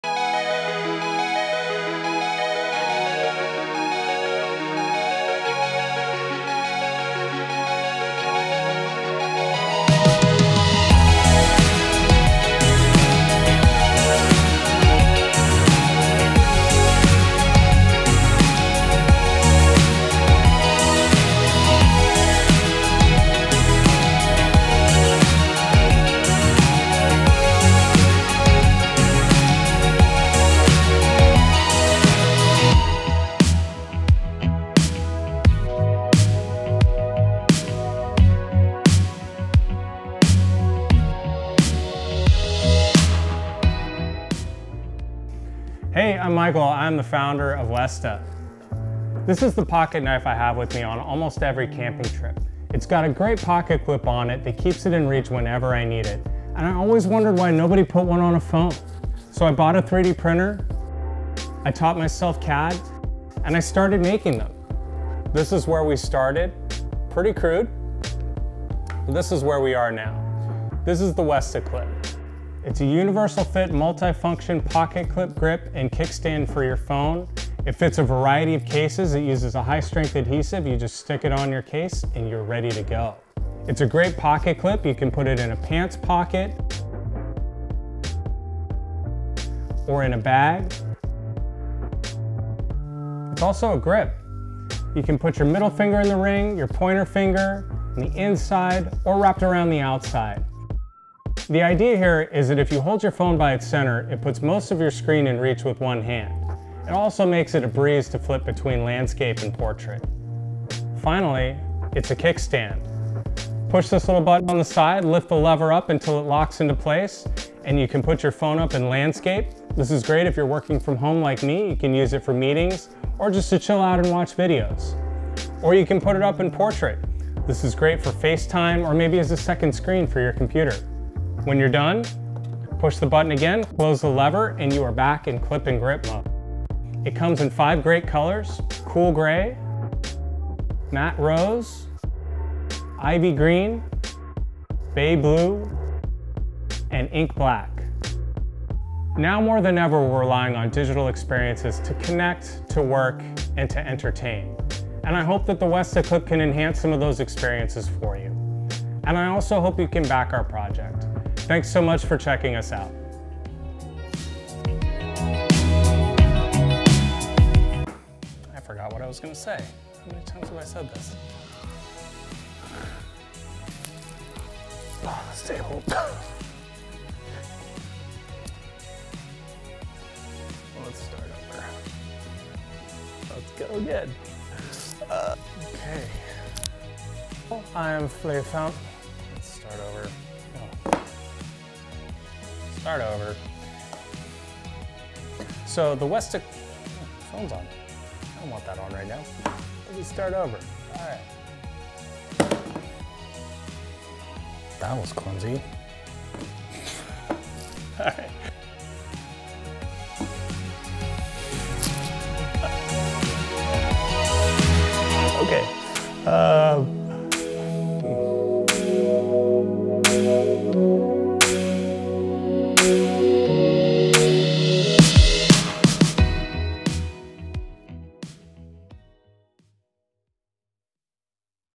The Westa Clip
We took the humble pocket clip and added smart features to solve all the ways you want to carry your phone. The ring gives you a solid one-handed grip on the go, lying down, sitting up, upside down, playing games, shooting video, or taking selfies. Landscape or portrait. Reach every corner of your phone with one hand without adjusting your grip. The Clip holds tight to your pockets or your bag. Your phone is always in fast reach. It's also a kickstand with a slick all-metal mechanism to hold your phone in portrait or landscape. Crafted from exceptional quality aluminum and EMS Grivory composite.